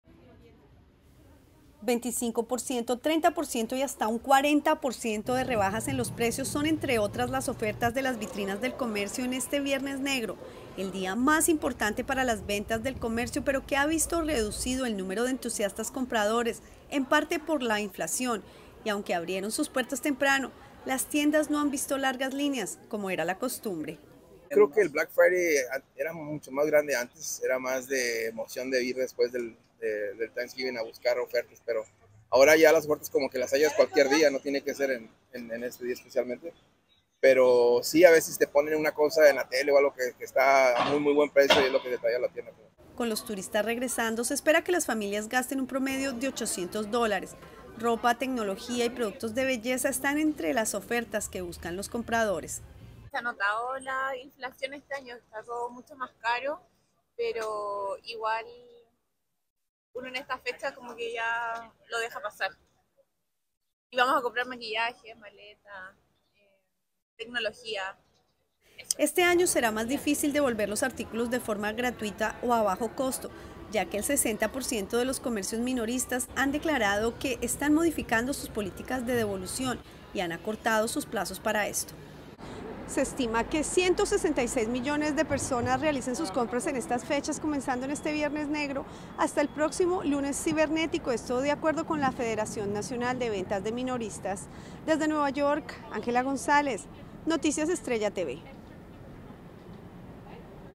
Así se vivió el Black Friday en Nueva York
🇺🇸 Con rebajas de hasta el 40 por ciento las vitrinas de la "Gran Manzana" esperaban a miles de personas para este Black Friday. Sin embargo, la inflación golpeó el bolsillo de las personas.